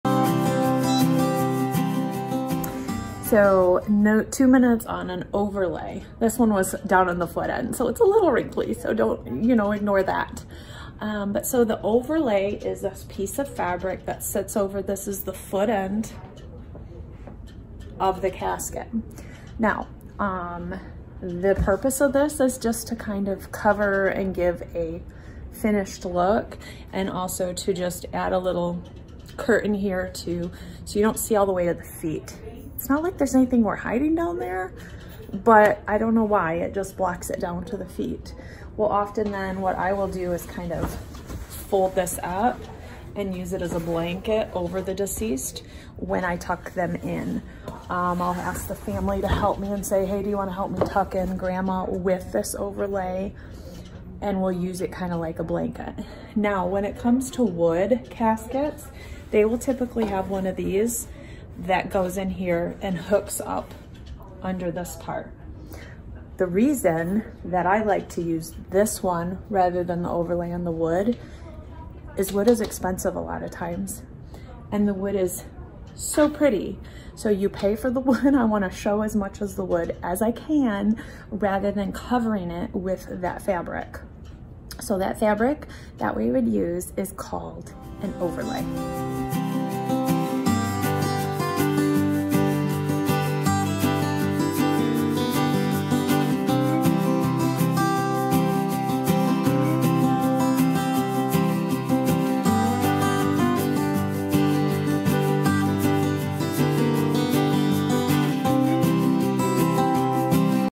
What is a Casket Overlay? -Just Give Me 2 Minutes
What is a casket overlay? Kari the Mortician explains what a casket overlay does and what caskets have them.